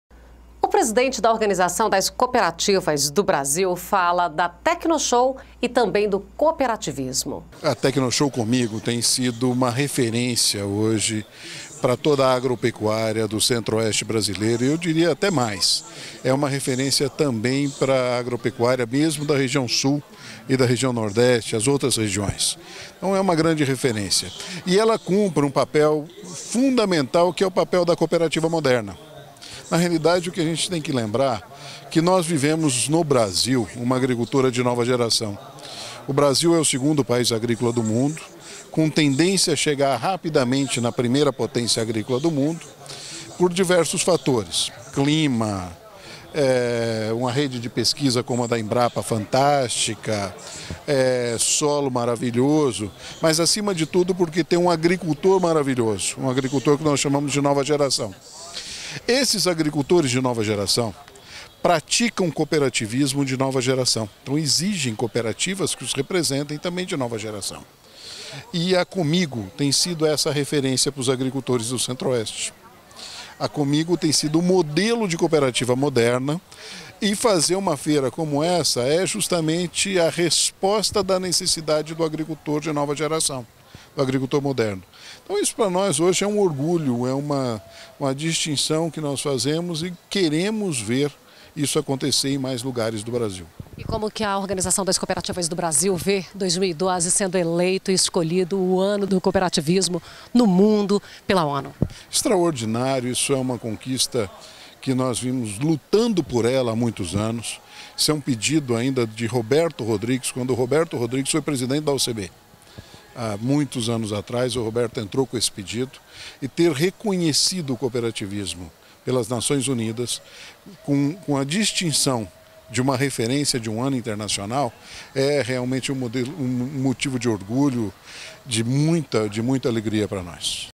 Presidente da OCB fala da Tecnoshow
Presidente da Organização das Cooperativas do Brasil fala da Tecnoshow e do Cooperativismo